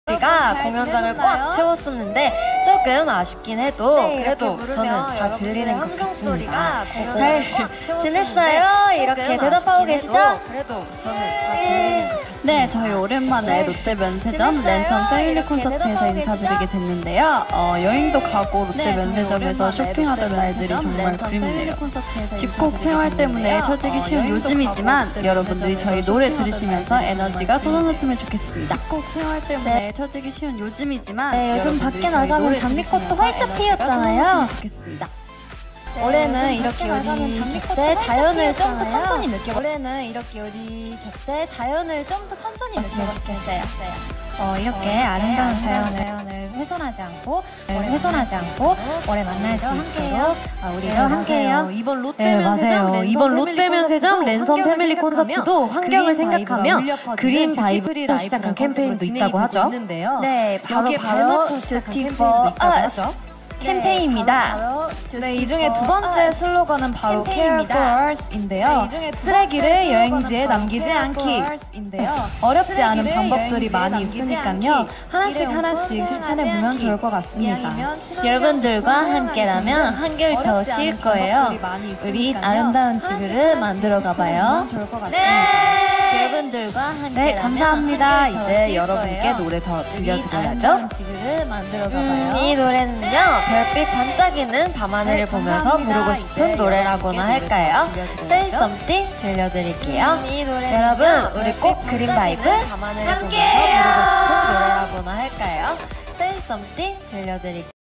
TWICE being eco friendly to earth(Lotte duty-free family concert)
16/5/21 Lotte duty-free family concert, they are sweet!